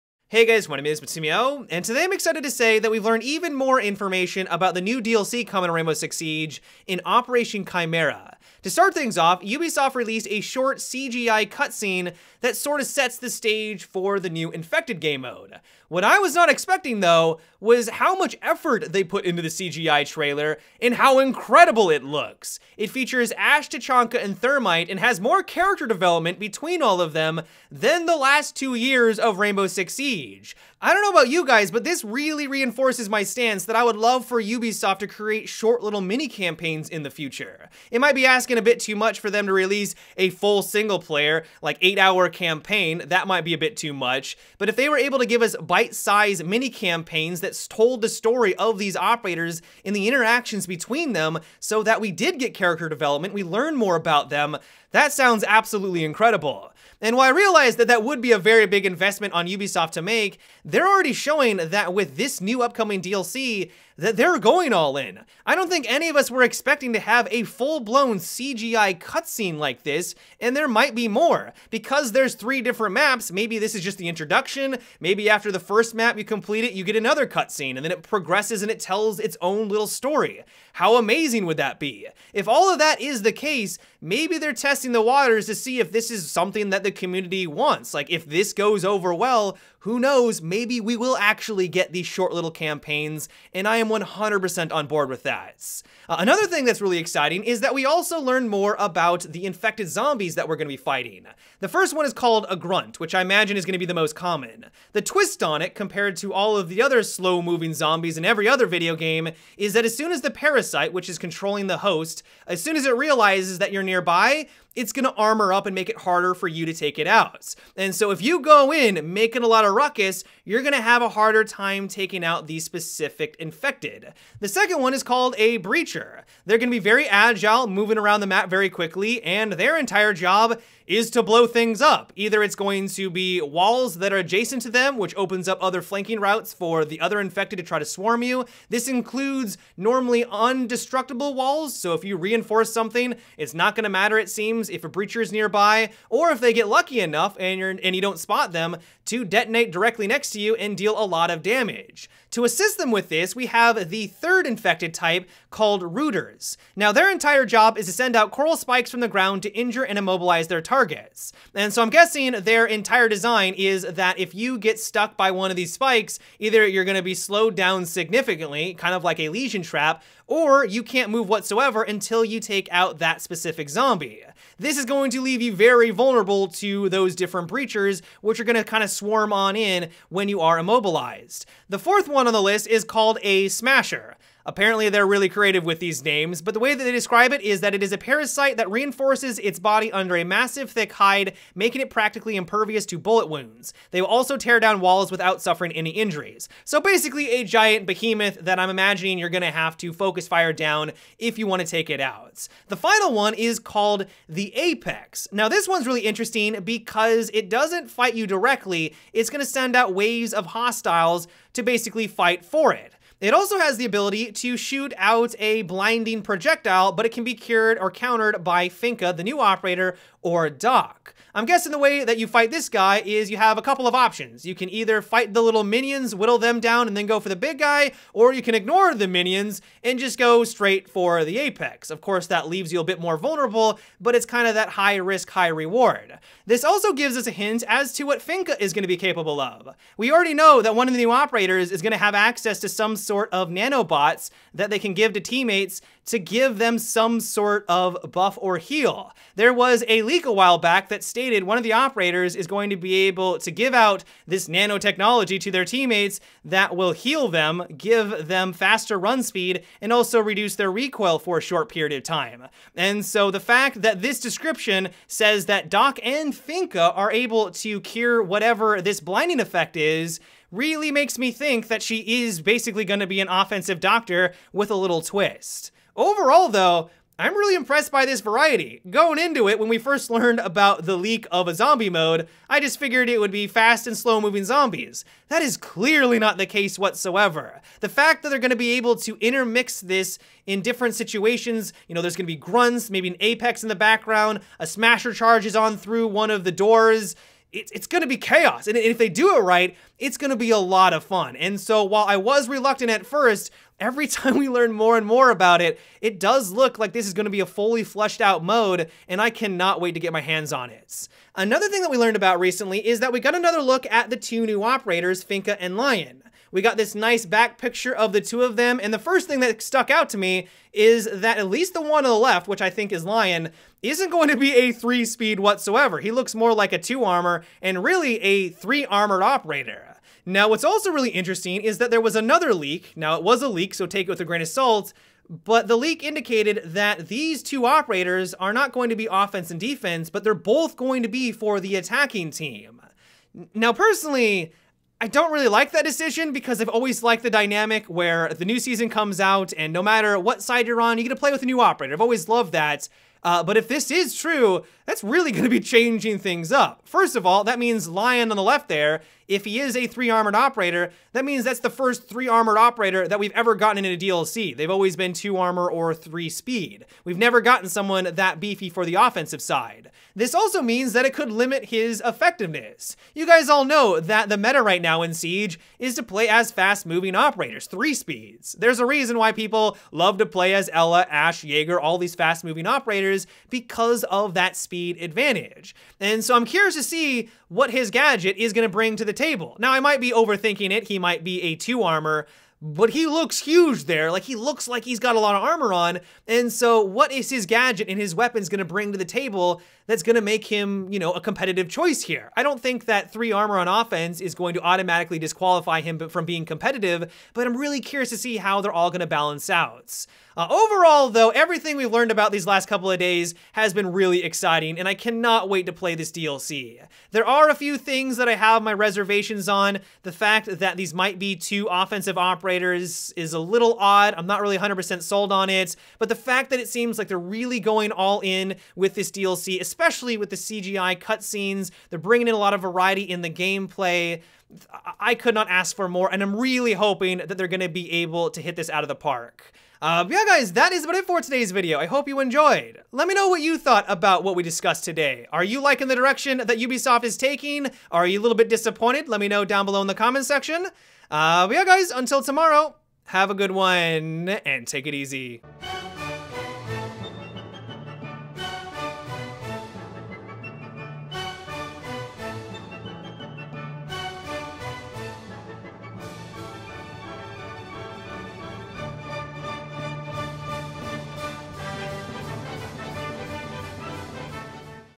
Year 3 DLC Update - Rainbow Six Siege
Today we learn more about the new DLC coming to Rainbow Six Siege.